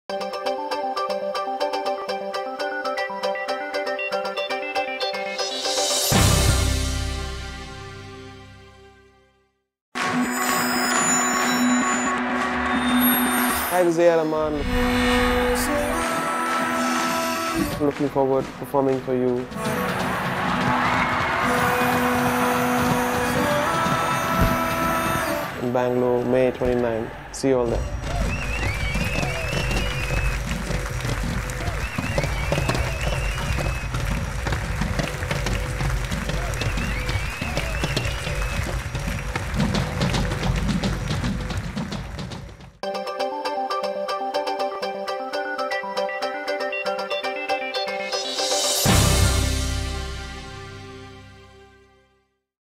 AR Rahman
A.R.Rahman Live in Concert - May 29, 2011, Palace Grounds, Bangalore -- that's the mantra for musical nirvana! After all, it's not every day that you get to witness a musical extravaganza of this magnitude, A R Rahman Live in Concert' will not only have a majestic stage and enthralling performance but will also include a unique feature with AV Screens across palace grounds showcasing the concert in 3D. Yes you heard right 3D. All this can only happen when two giants join hands. A.R.Rahman and the UF Group.